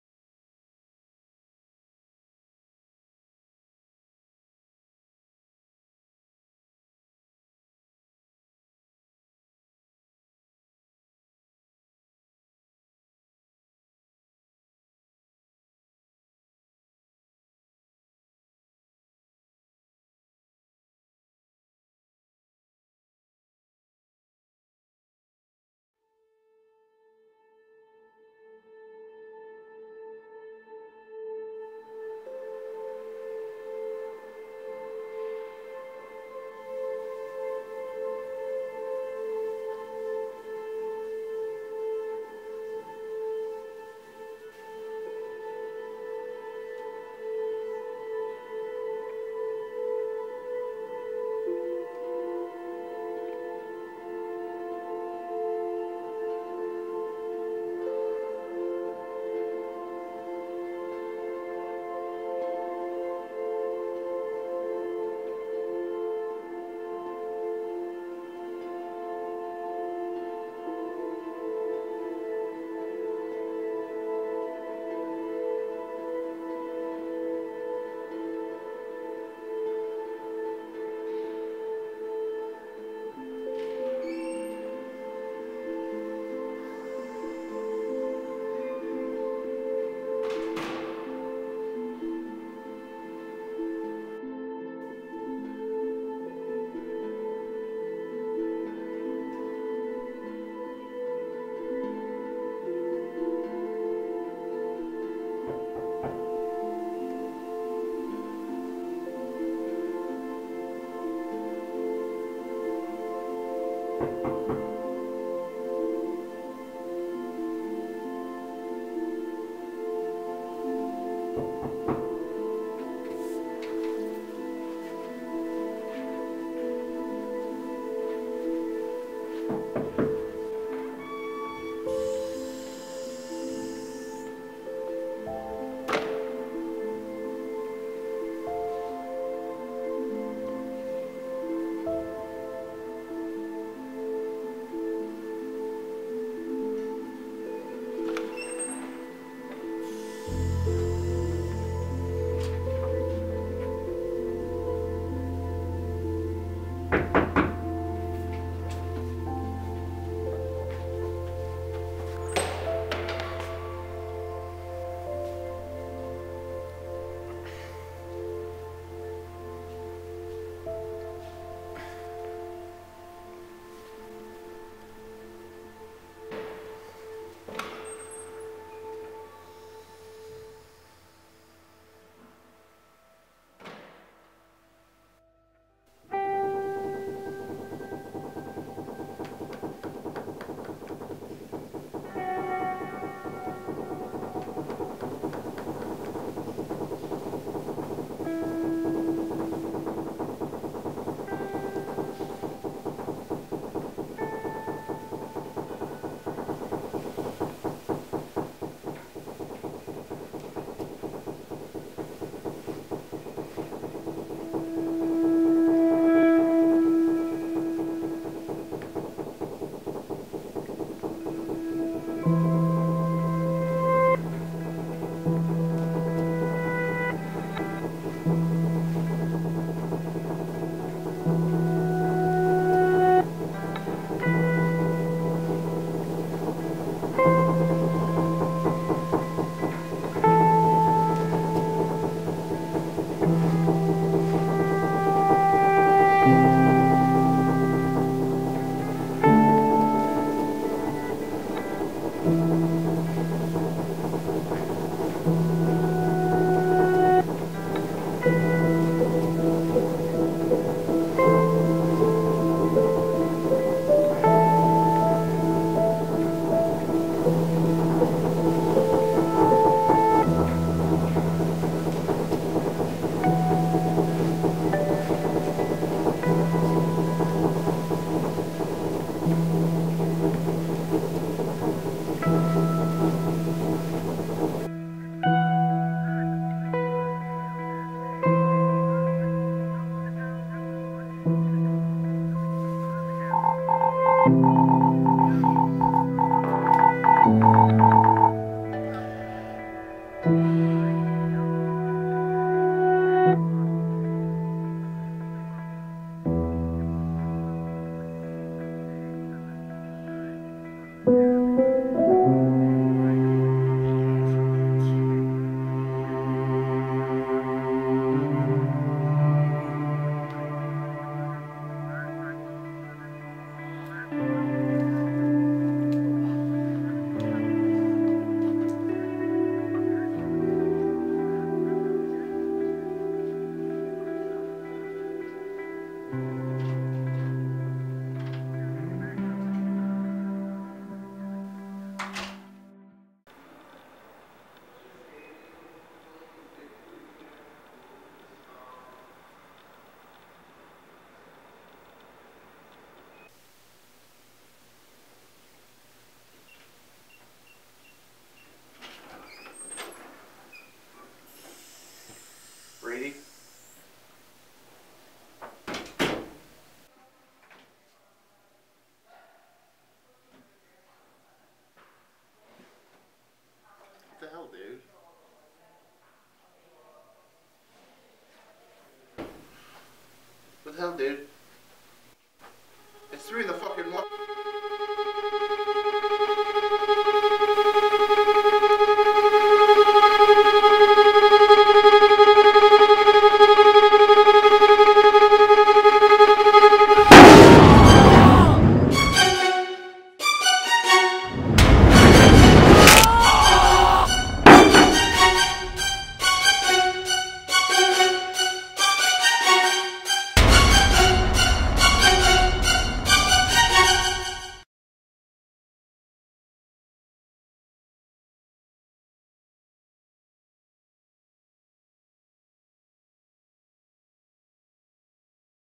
The Valley Incident
On one cold lonely night at SVSU a young radio DJ is going to experience something not of this Earth.